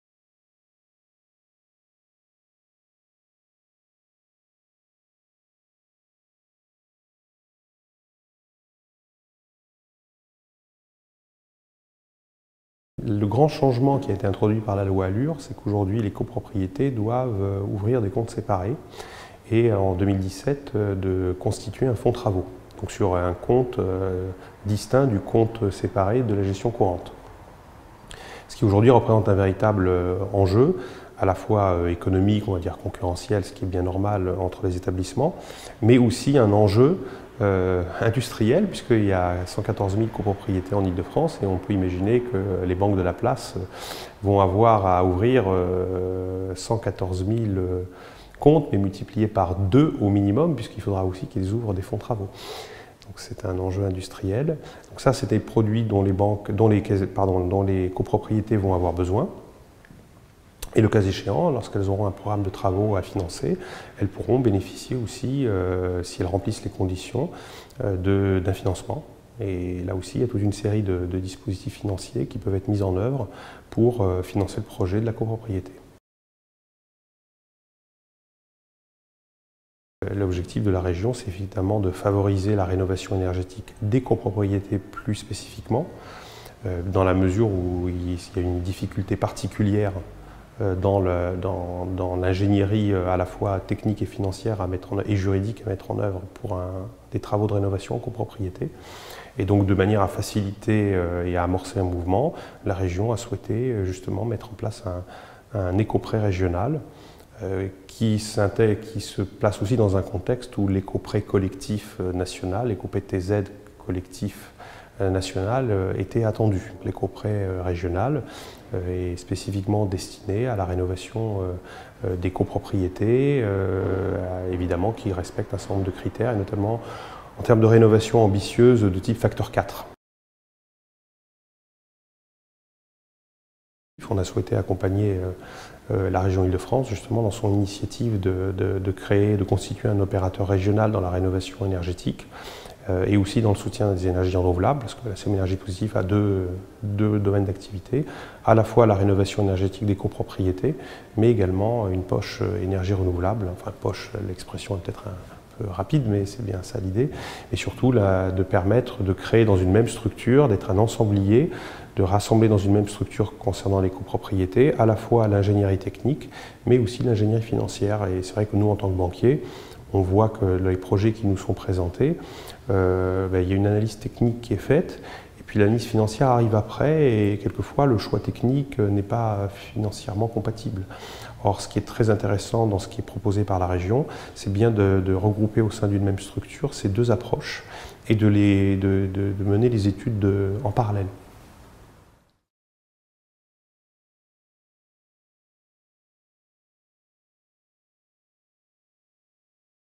Rénovation énergétique, le regard du banquier
A l'occasion de la journée "Financer la rénovation énergétique des copropriétés" organisée conjointement par l'ARENE et la SEM Énergies POSIT'IF, l'ARENE a interrogé Serge Tisserant, responsable développement durable de la Caisse d'Épargne Île-de-France, sur les outils financiers franciliens à la rénovation énergétique des copropriétés. Il revient sur la loi ALUR, l'appel à manifestation d'intérêt de la Région Île-de-France sur un écoprêt pour la rénovation énergétique des copropriétés franciliennes et sur la SEM d'ingénierie technique et financière Énergies POSIT'IF dont la Caisse d'Épargne Île-de-France est partenaire.